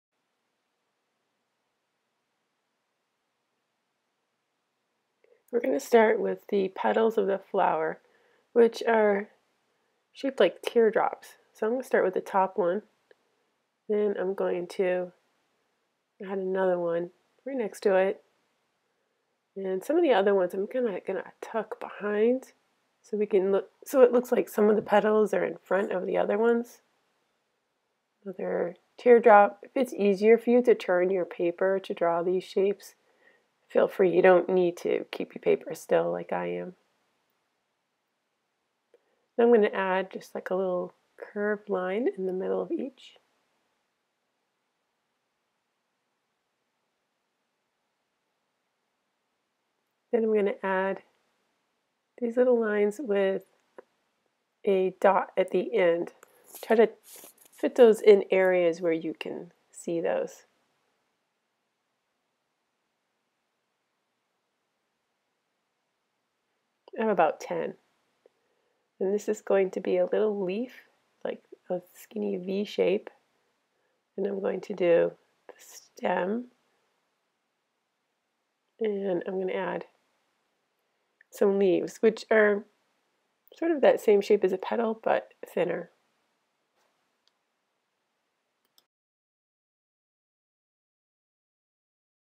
How to draw a lily
Follow step by step to draw a lily flower.